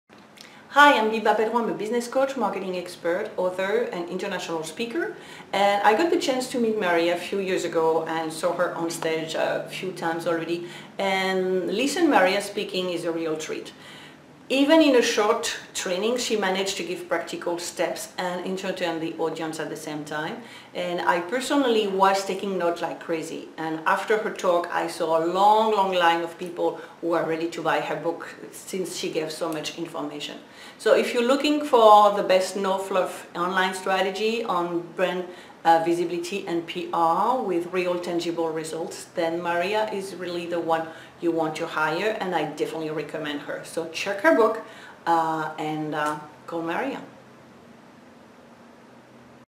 Biba Pedron review about Maria Serbina speaking
Biba Pedron review about Maria Serbina speaking at Hell Yeah X-event (TED talk like) in Maimi, FL.